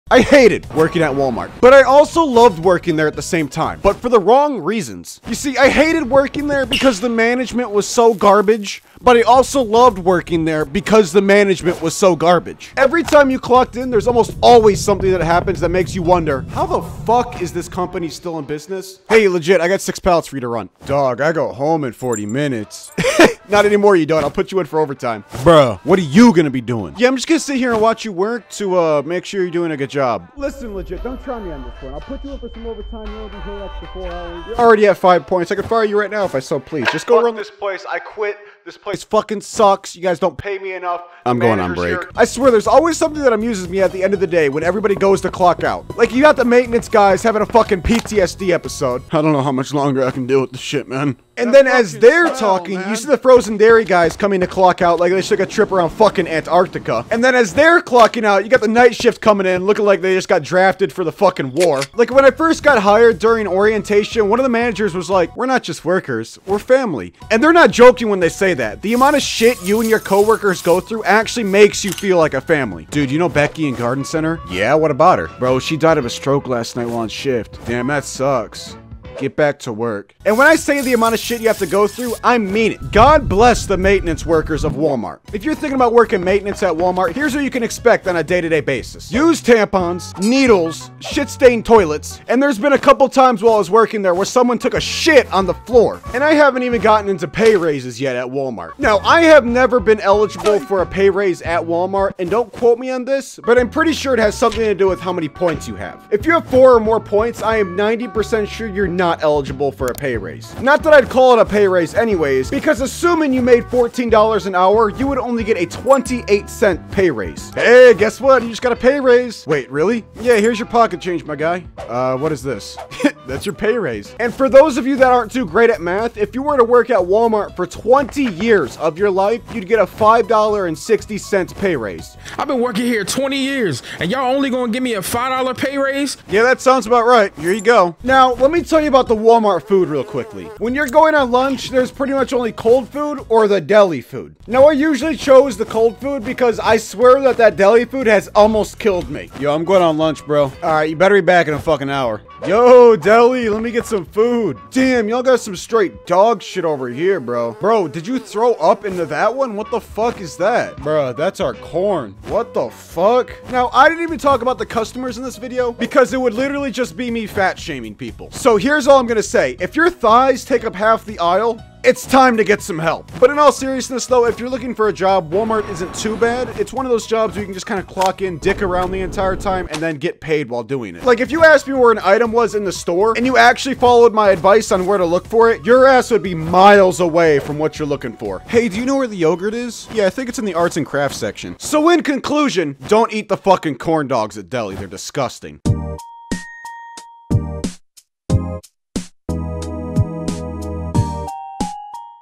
Why Walmart Sucked - Animated Story
Walmart is a circus with both thee employees and customers being the main attraction.
These Walmart videos don't get much views but talking and animating Walmart is something I specialize in.

Walmart if you see this video just know I'm done talking about your company